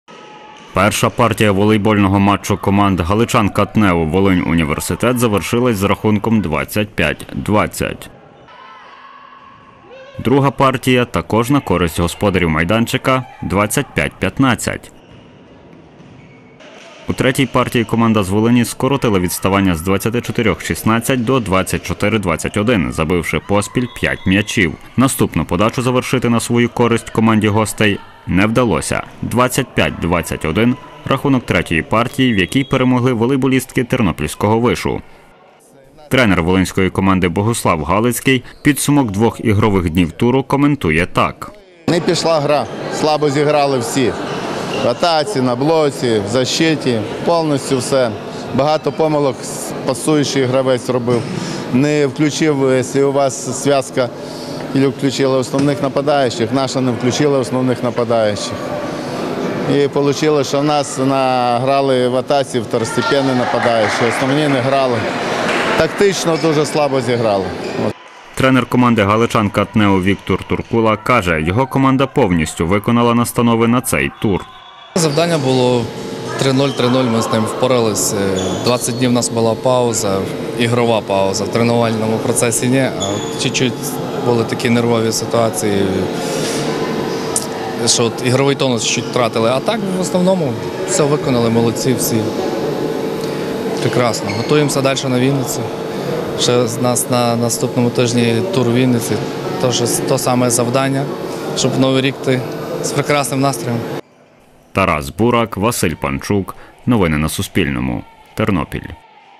"Галичанка-ТНЕУ" перемогла "Волинь-Університет" у волебольному матчі з рахунком 3:0
З рахунком 3:0 на користь тернополянок завершилась друга гра сьомого туру жіночої супер-ліги з волейболу між командами "Галичанка-ТНЕУ" та "Волинь-Університет". За підсумками двох ігор туру тернополянки перемогли волинську команду з рахунком 6:0. Повідомив тренер команди "Галичанка-ТНЕУ" Віктор Туркула.